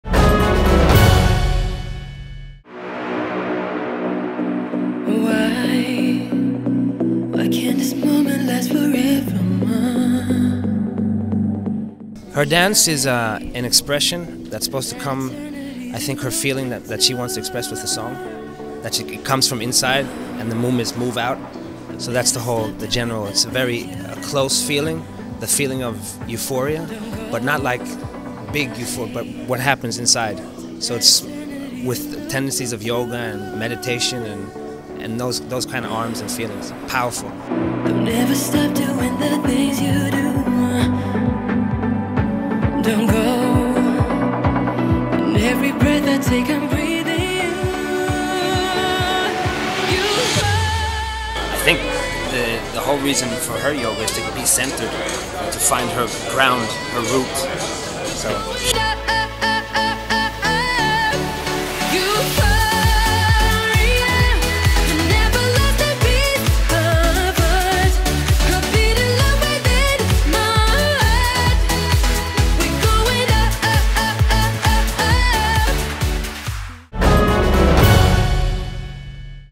Loreen reveals the reason for yoga elements in her stage show
Rennie Mirro spoke about the philosophy of dance.
Rennie Mirro, the choreographer of the Swedish representative Loreen, gave an interview to eurovision.az. He spoke about the philosophy of dance and use of yoga elements in Loreen's performance in Baku.
eurovision.az